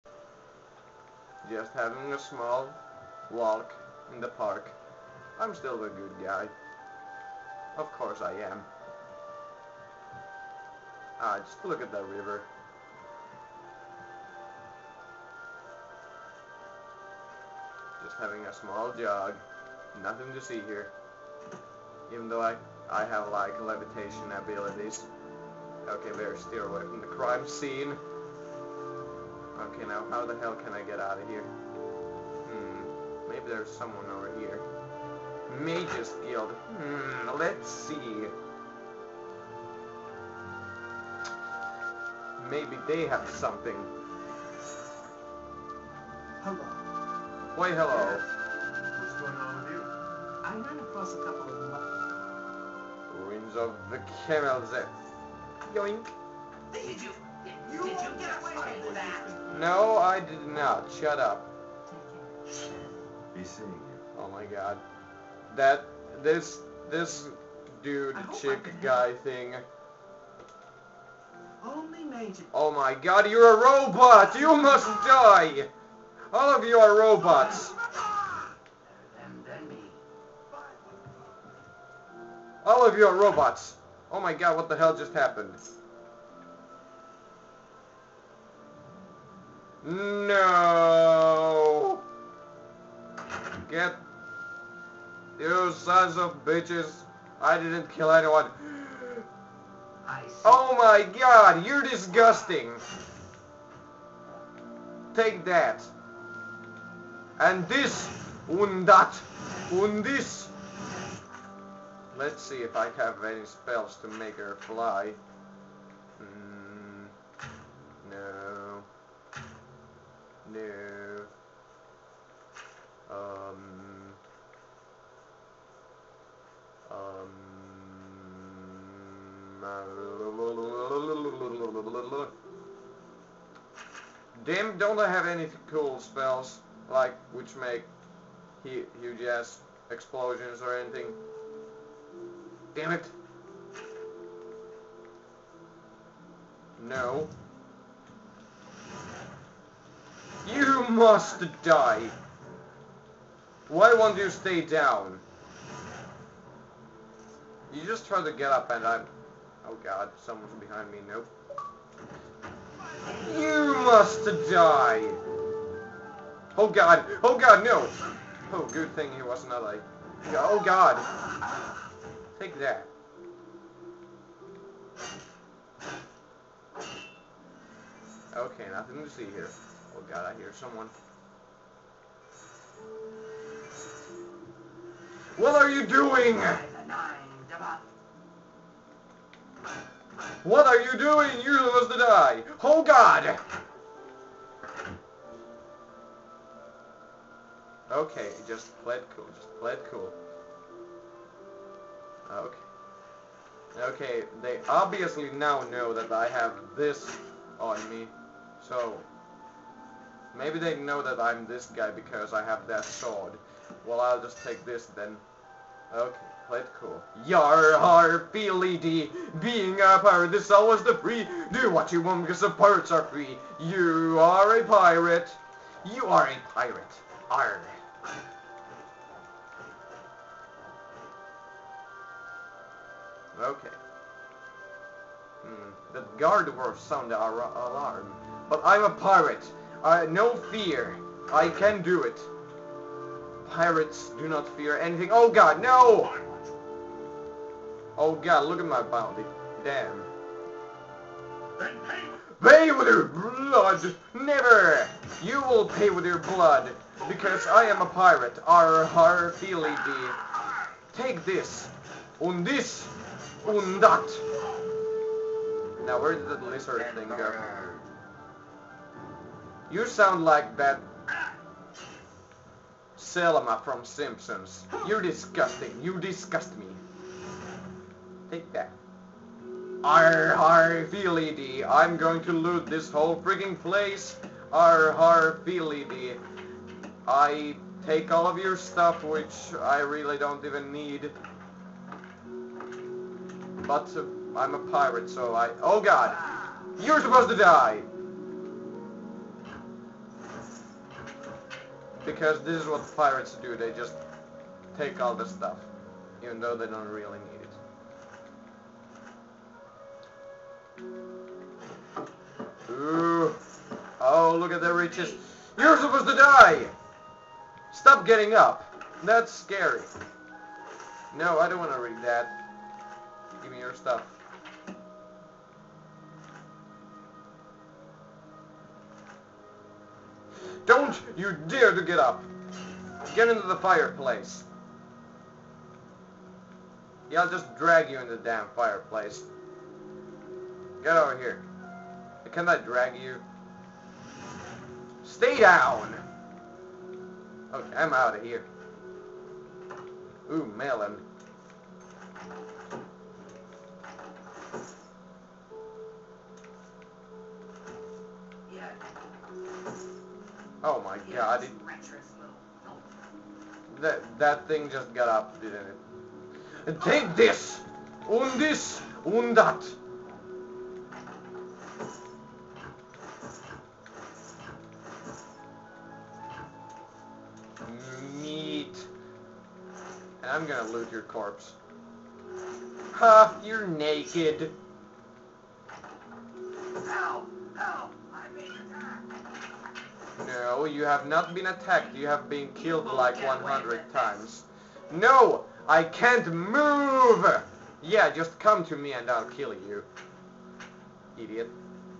let's pay with my blood pt.2 oh god the guards are on to me!
this was going to be a let's try, but then everything went hell in a handbasket real fast!